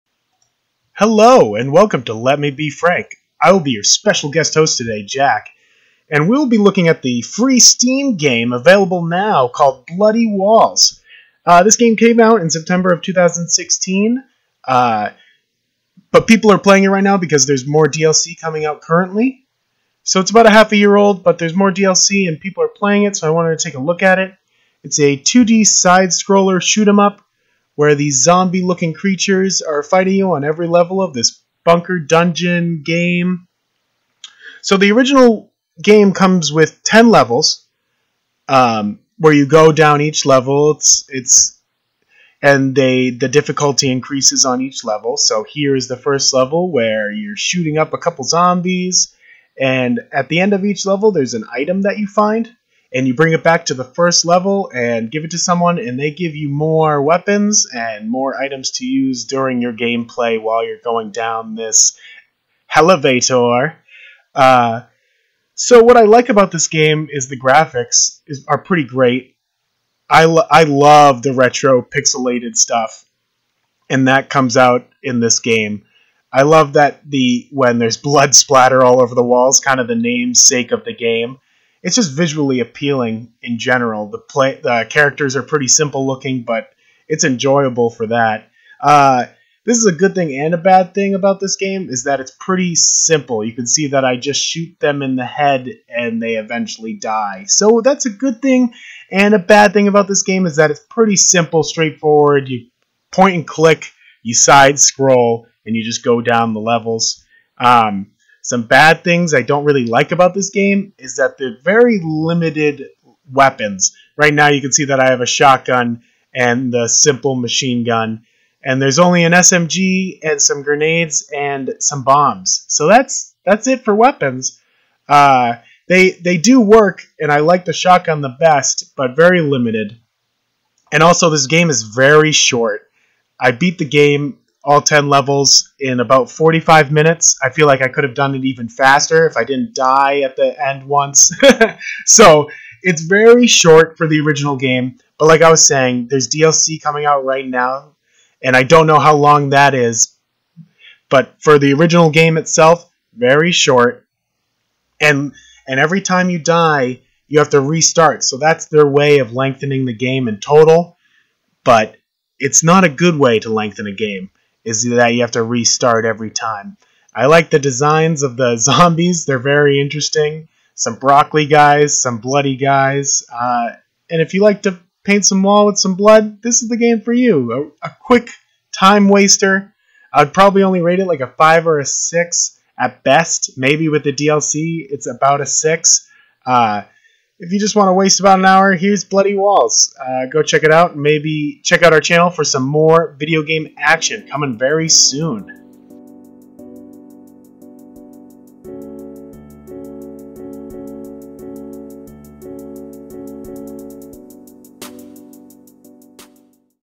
BLOODY WALLS - Let Me Be Frank Review (ft. JACK)
Jack is your special guest host on this episode of Let Me Be Frank.
He takes a quick look at the free Steam game, "Bloody Walls".